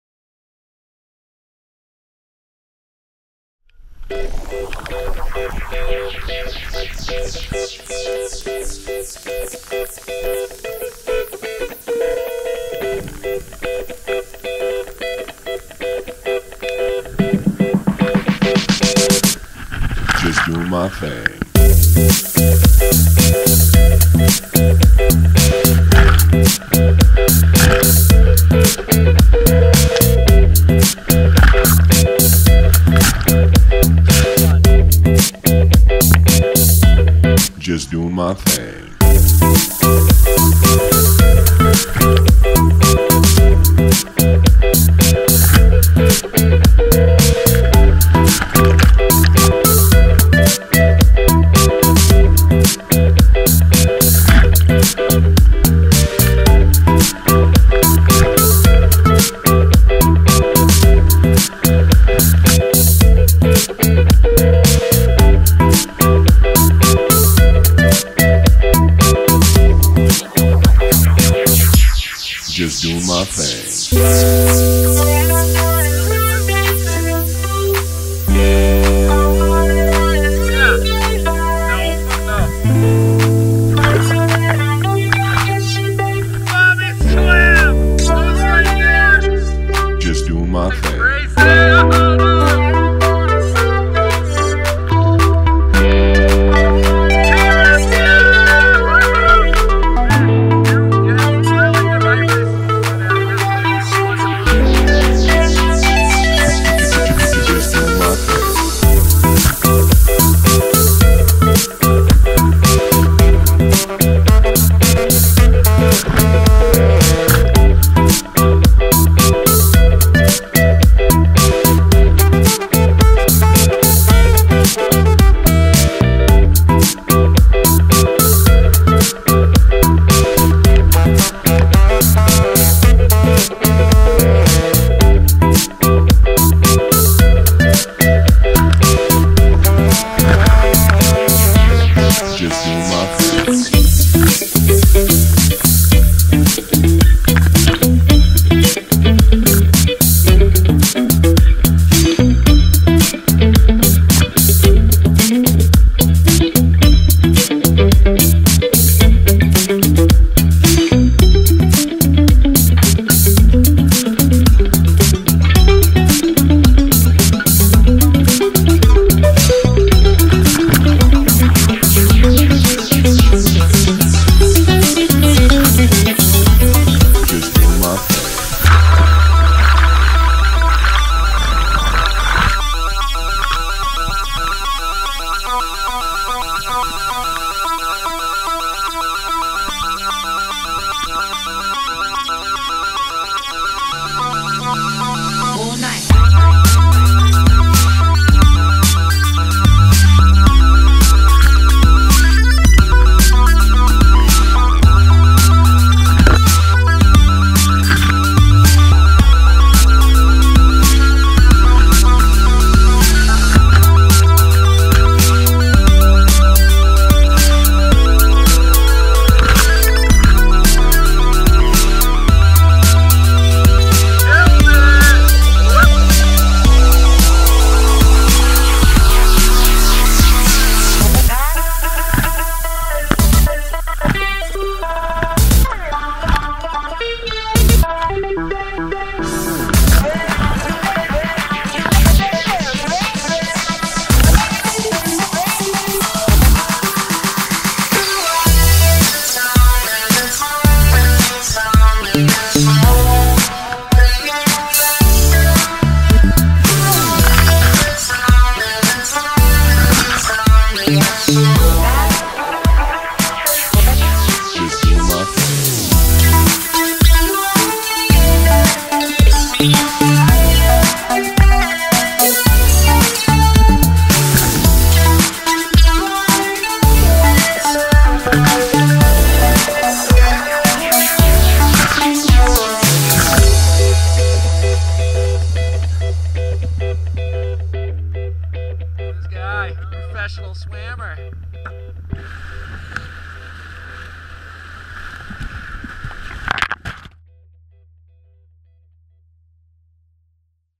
VST - White Water Weekend (Dec 5 + 6 2015)
VARSITY SWIM TEAM! Middle white salmon and lower white salmon laps on Saturday, Upper Clackamas on Sunday. Didn't capture much great footage on MWS/lWS due to dead gopro battery.